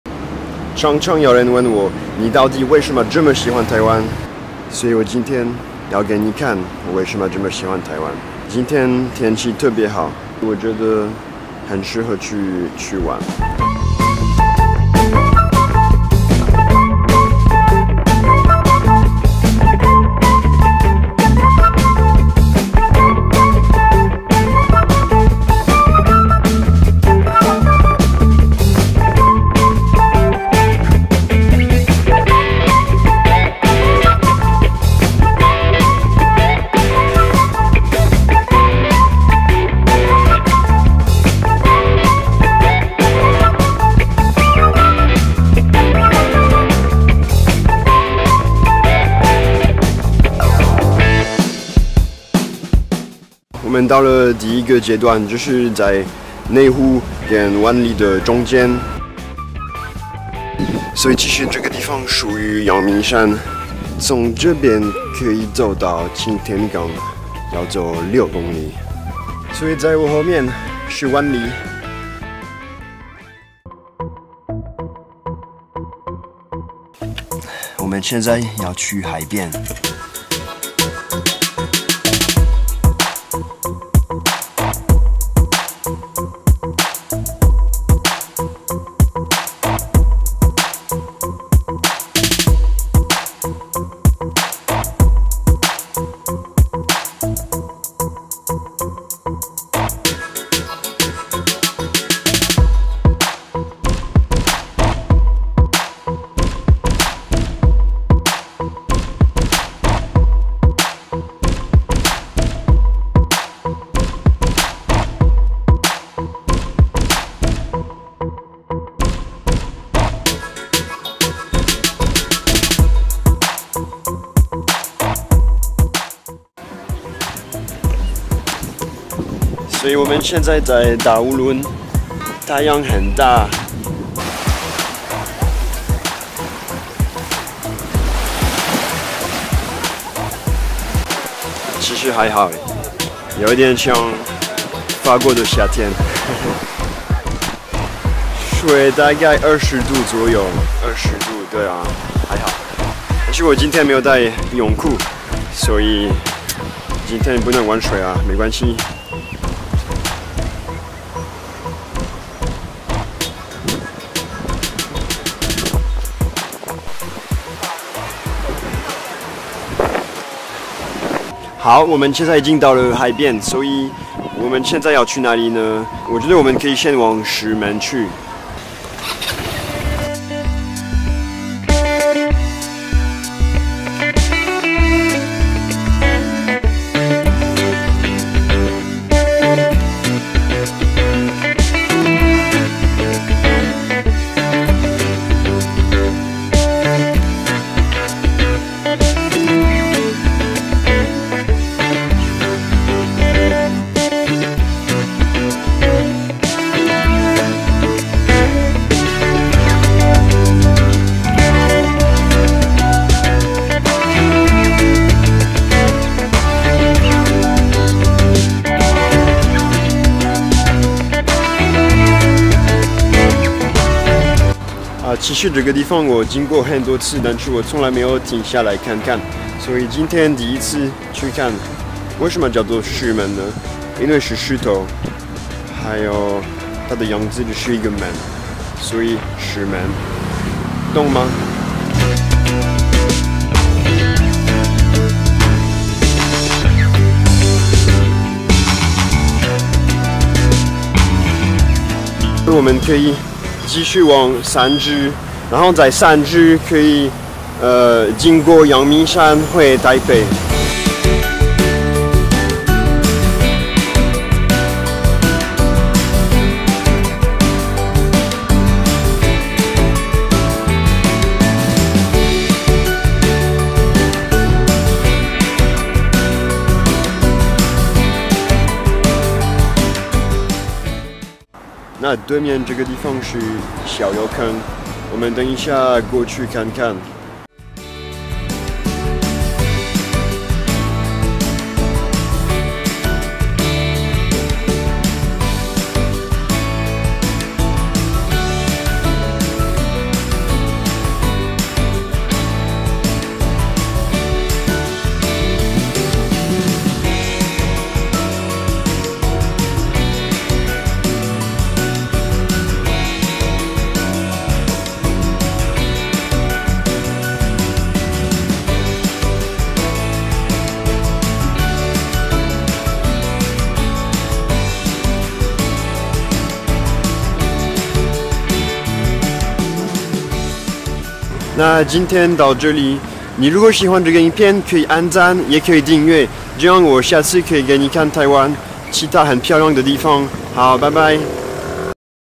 週末去哪裡玩？大武崙、石門、陽明山
這週末天氣超好，本來要去咖啡廳看書，但是結果決定要去海邊玩：）
於是我從台北出發往內湖騎機車。到內湖的大湖公園透過大湖街，往山上騎。路上很多小咖啡店，好可愛。我在大武崙吃午餐，玩一下水，然後往石門繼續。石門很多家庭在那邊玩水，因為那邊很多小水窪，裡面有魚，螃蟹，小朋友玩得開心。最後我去了陽明山的小油坑，還有在文化大學看日落。